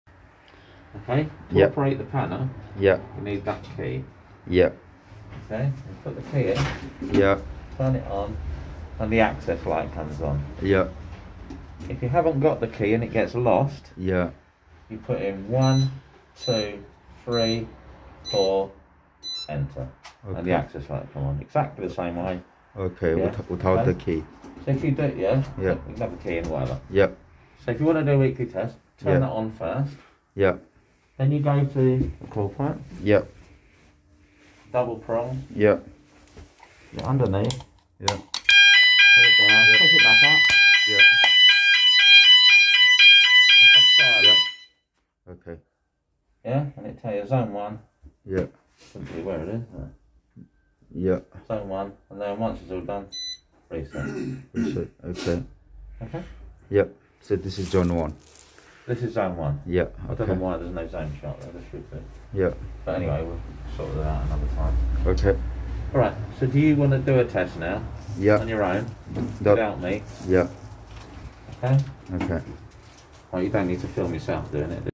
Haes fire alarm panel
Quick video on Haes fire alarm panel system. This was fitted in one of our HMO 7-bed house (4 zones)

This panel is very easy to conduct the weekly test and compact